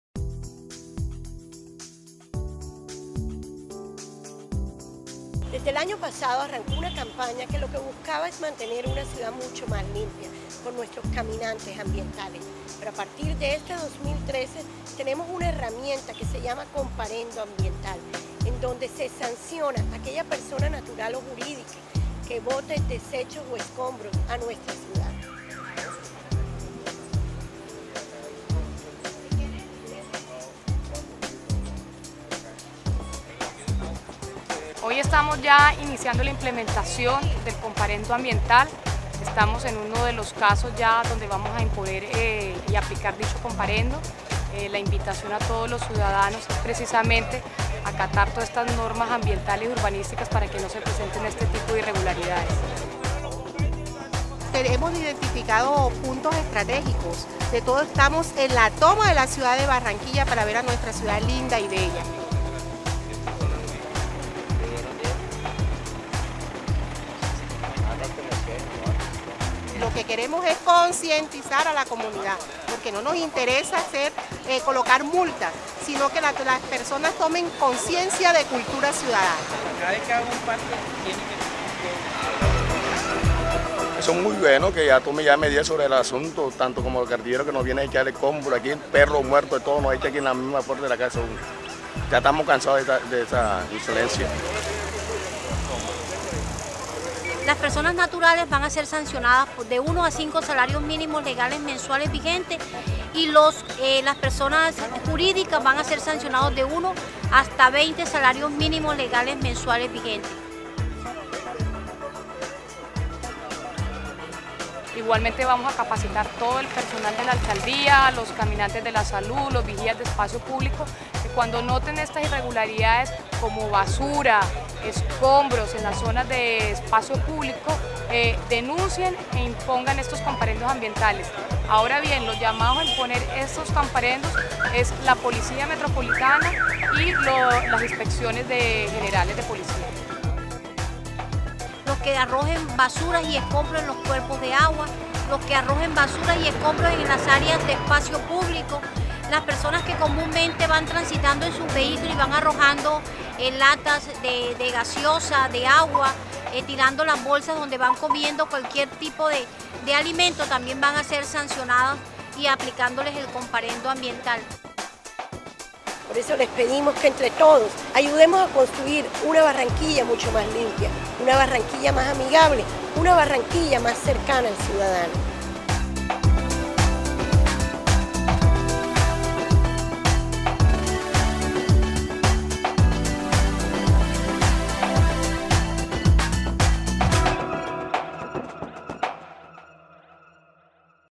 Comparendo Ambiental en Barranquilla
Desde el 18 de marzo de 2013, comenzó la implementación del Comparendo Ambiental en Barranquilla. La Administración Distrital lidera este tema por intermedio de las secretarías de Gobierno y Movilidad, que tienen a su cargo los procesos de las sanciones económicas a quienes contaminen en la ciudad el medio ambiente a través de residuos sólidos y escombros en sitios no autorizados. En la Policía Ambiental, así como en los agentes de tránsito, los inspectores de Policía del Distrito y los corregidores, recae la competencia de imponer el Comparendo,